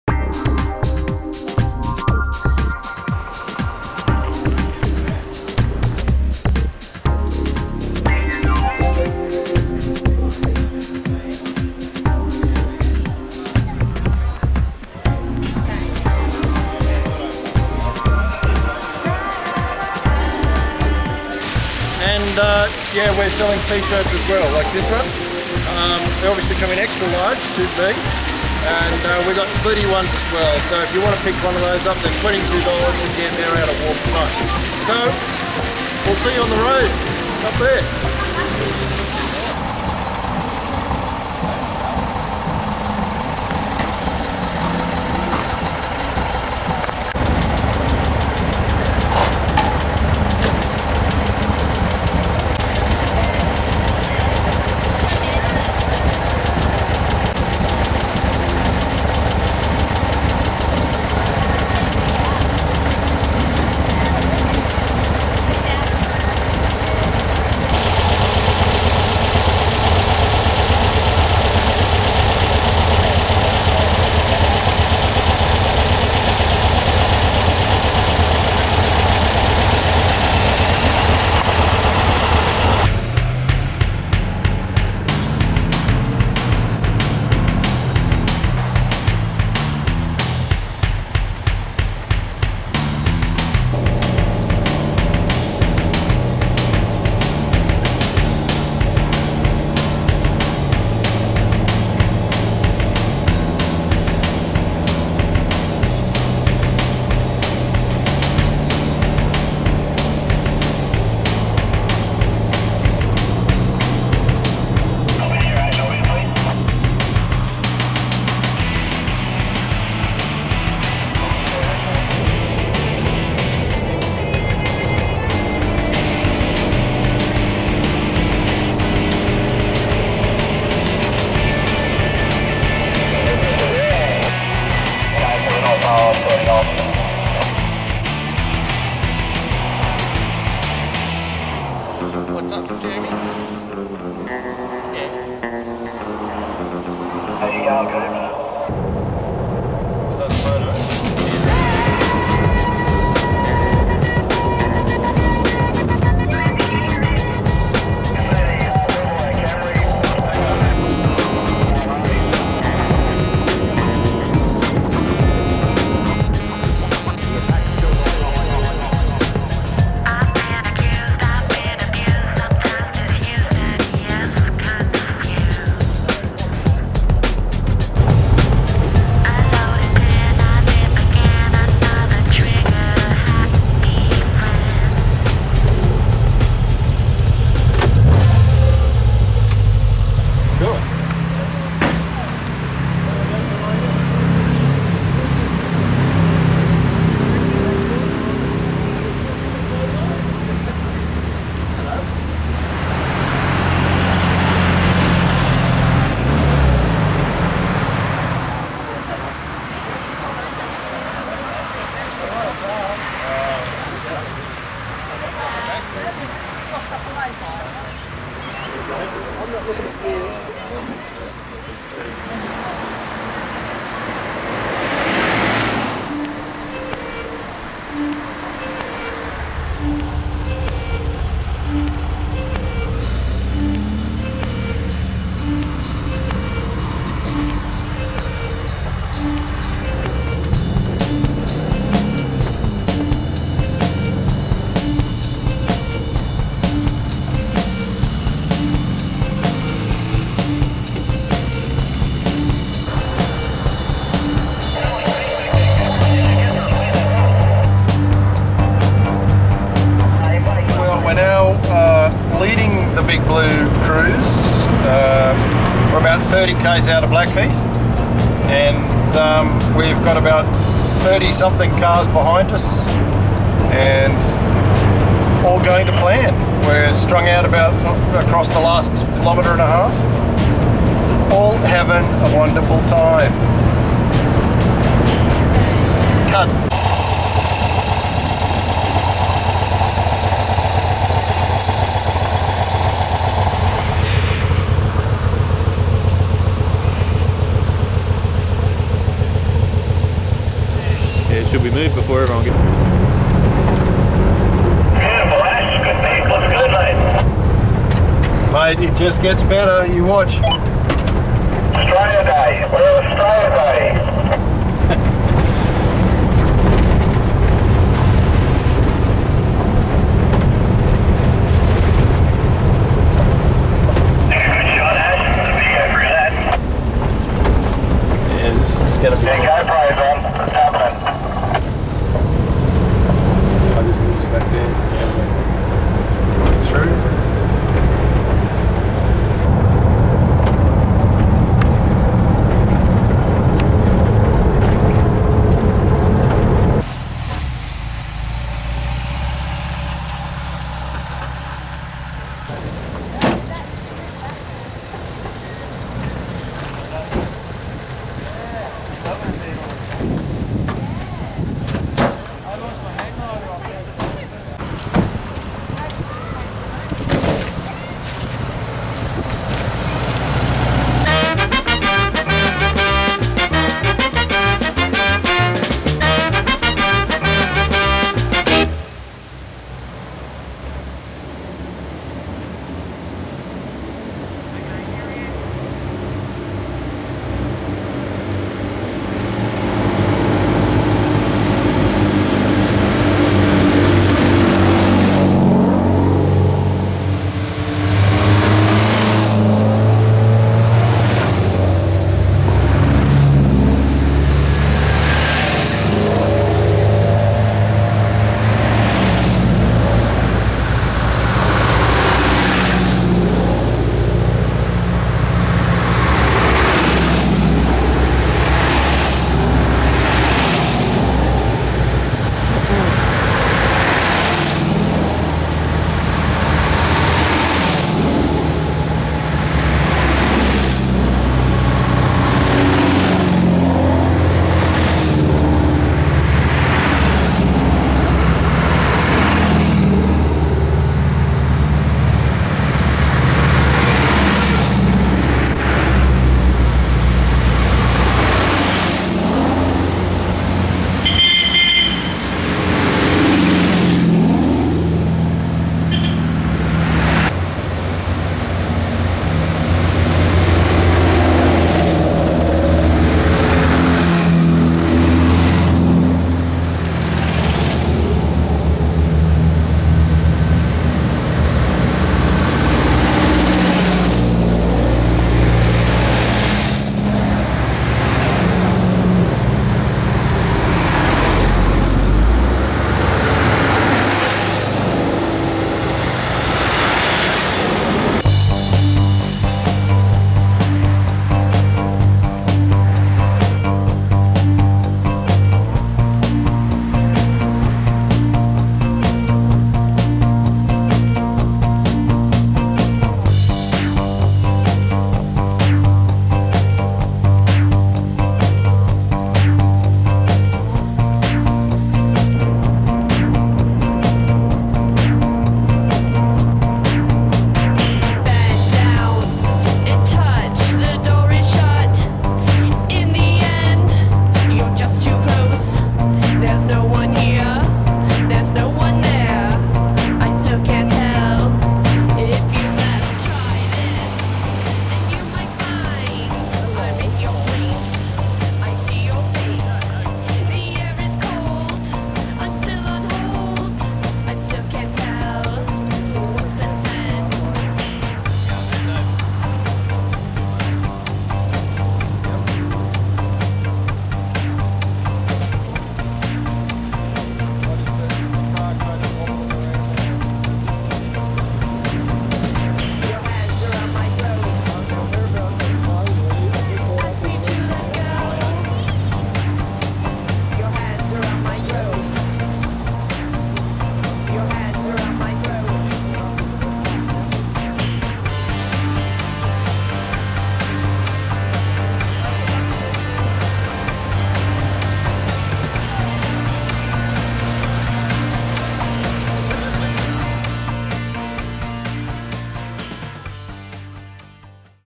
The Big Blue 2012 - VW cruise to the Blue Mountains
The Big Blue 2012, a cruise organised by Volks Culture, involved meeting at Rouse Hill and cruising up into the Blue Mountains of NSW, Australia. Turnout was excellent with 35 cars and buses along for the ride.